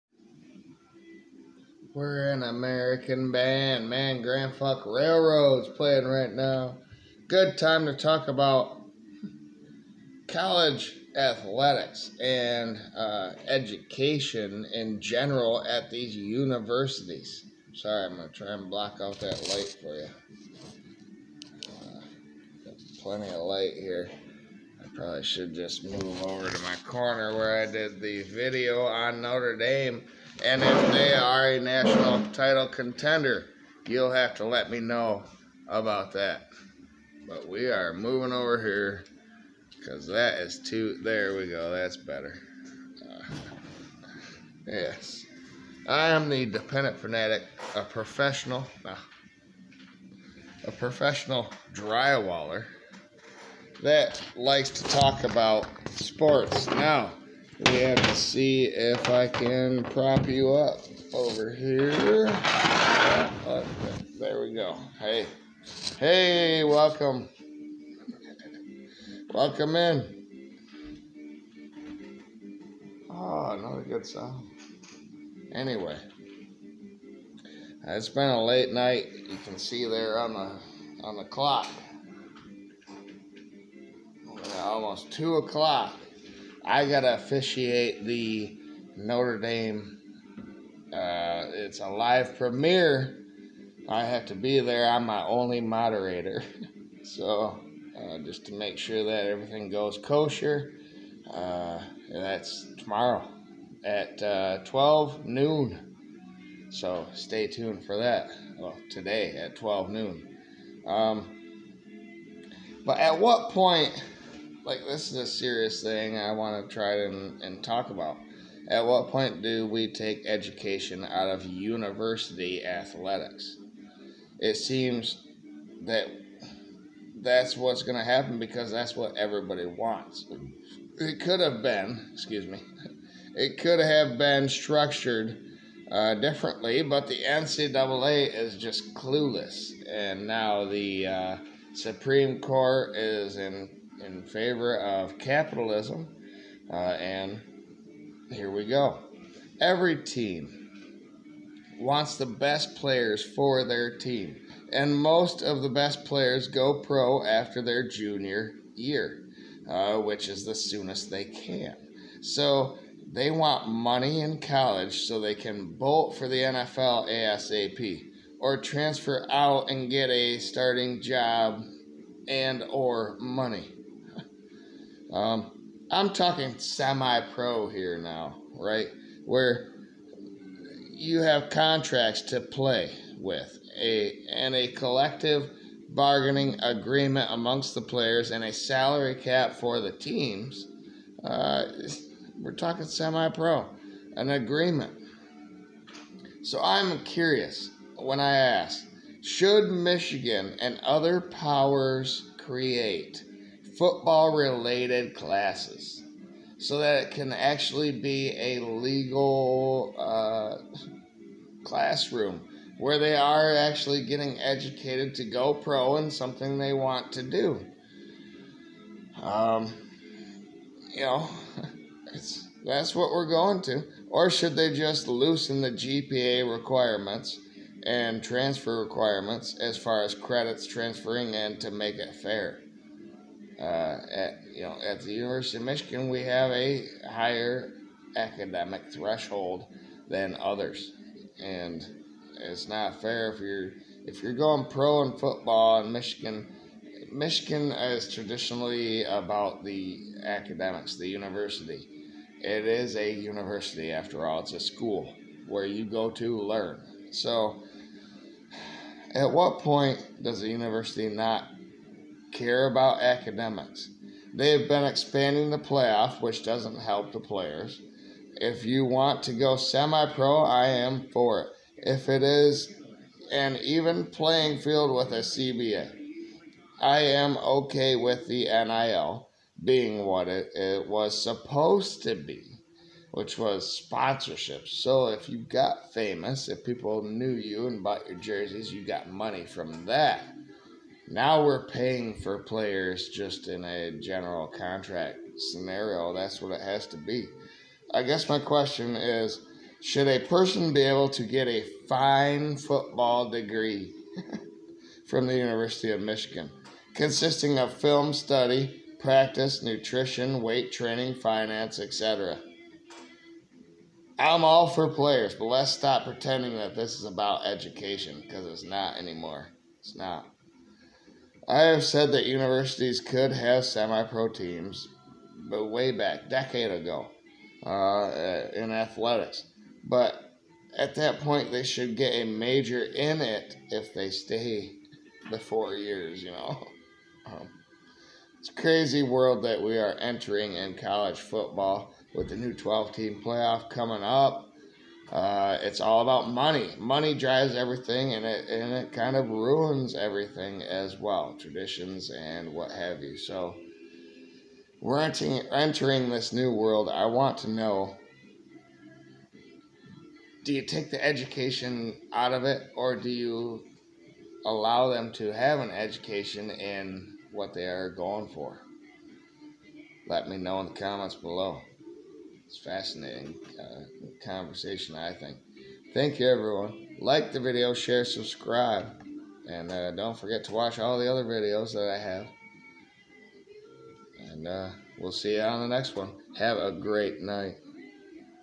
Should You be Able to get a College Football 'Degree'?
NIL has gone wild. So how do Universities keep it about Education yet maintain their 2nd money train, which is CFB?